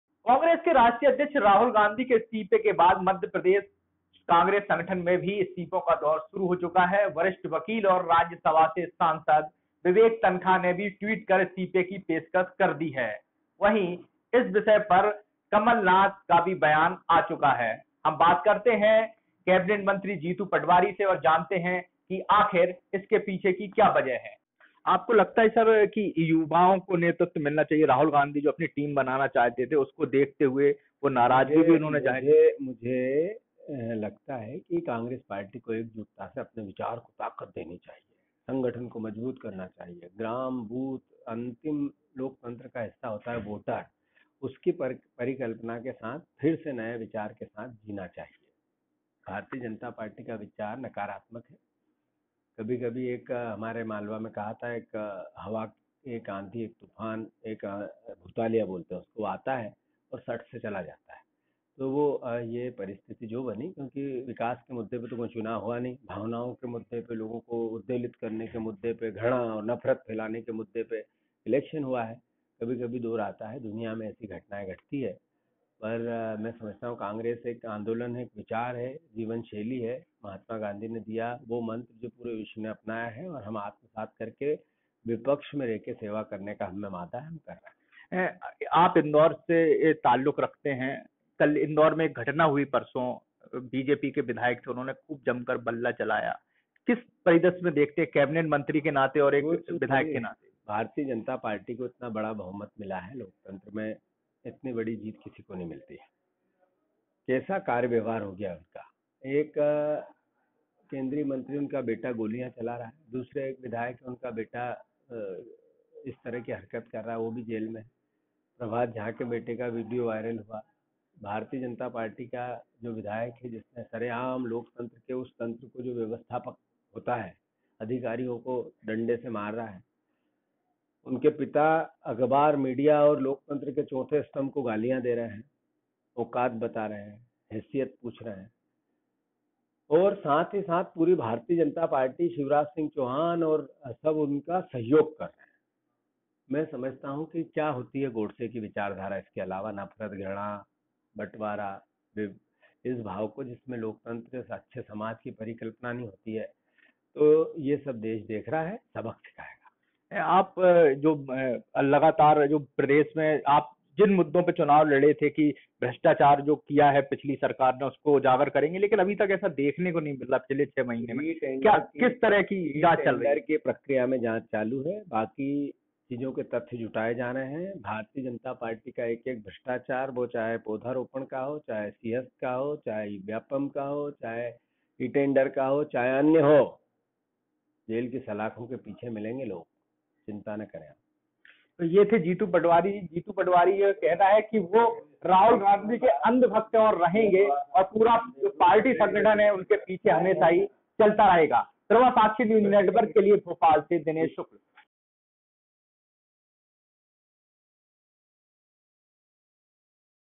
जीतू पटवारी ने कहा गोडसे की विचारधारा का पालन करती है भाजपा
मध्य प्रदेश सरकार में कैबिनेट मंत्री जीतू पटवारी ने प्रभासाक्षी के साथ खास बातचीत में कहा है कि भ्रष्टाचार मामलों में भाजपा के नेता जल्द ही जेल के अंदर होंगे। उन्होंने कहा कि भाजपा गोडसे की विचारधारा का पालन करती है जिसका विरोध किया जाना चाहिए।
जीतू पटवारी ने कहा गोडसे की विचारधारा का पालन करती है भाजपा - jitu patwari attacks bjp on kailash vijaywargiya son issue